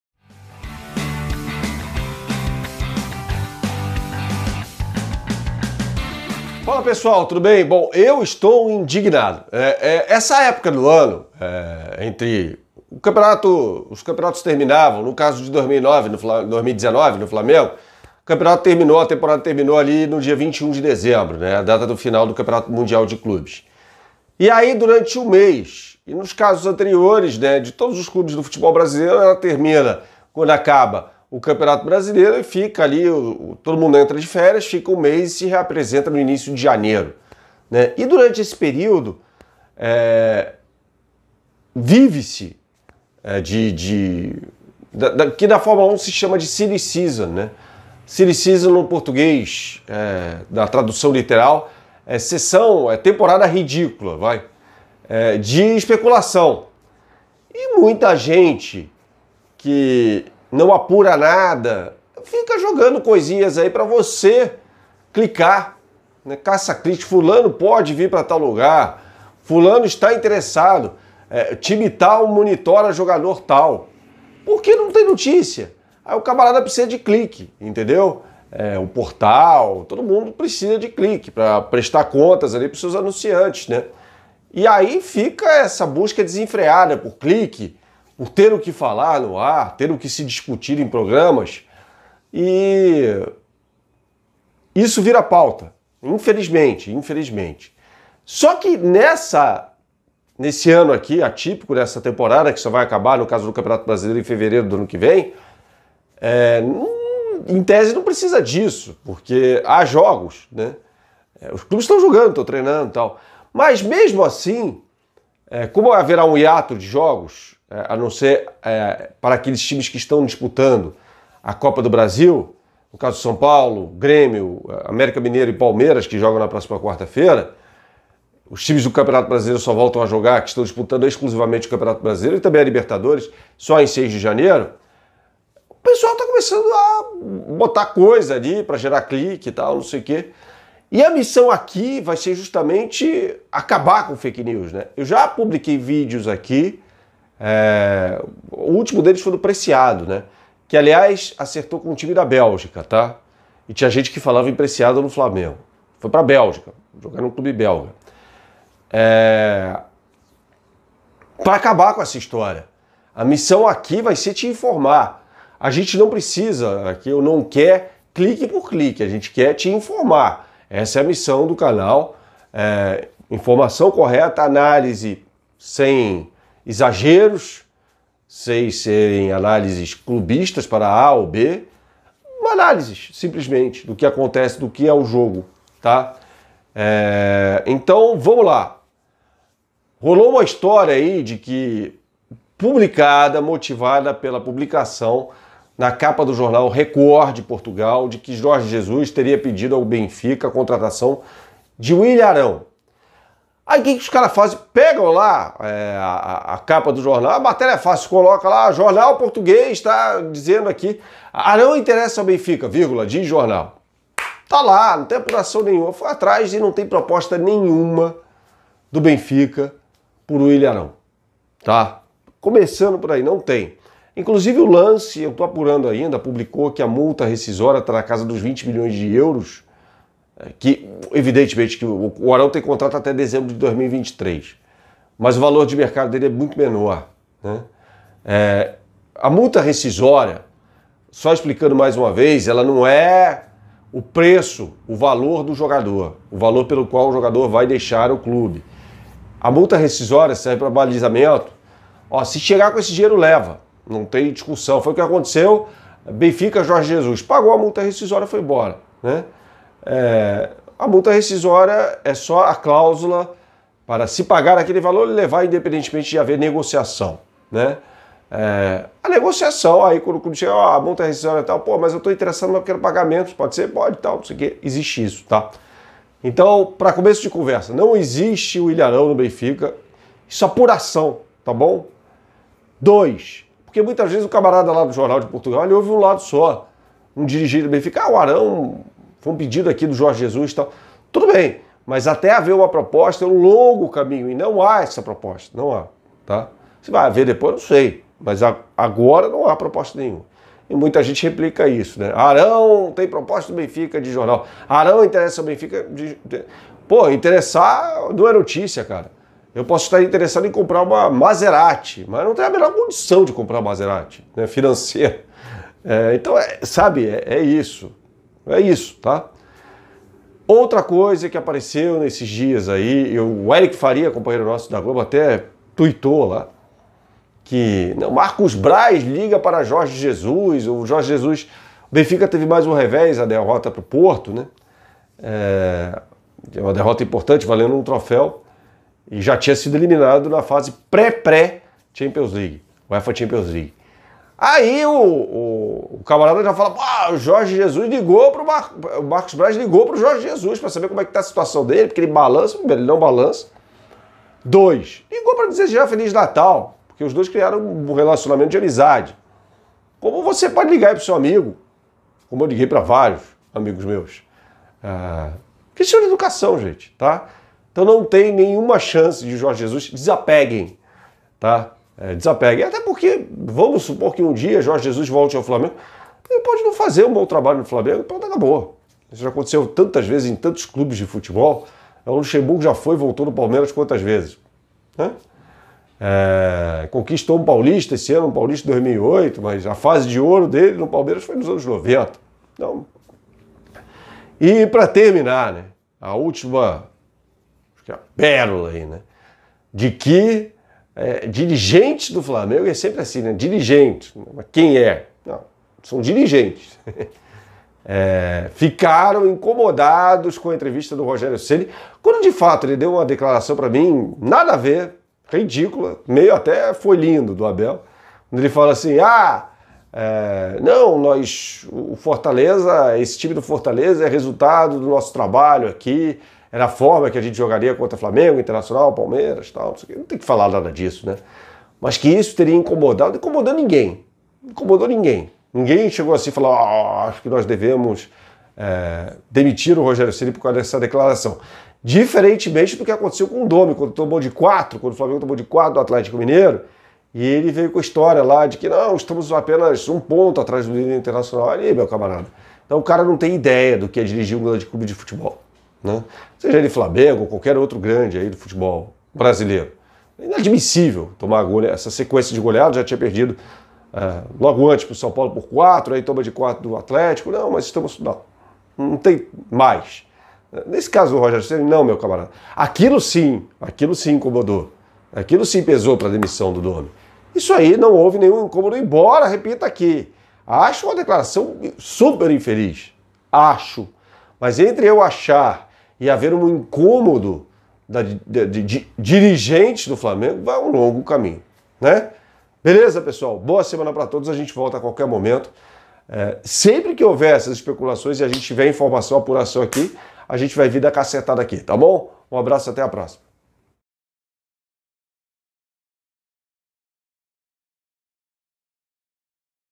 Arão não tem proposta do Benfica, Jorge Jesus não volta e ninguém ficou bravo com fala de Rogério
Na temporada em que não há jogos, houve reprodução e compartilhamento de notícias falsas, fake news no Flamengo.

Neste vídeo, contamos apenas a verdade sobre os casos.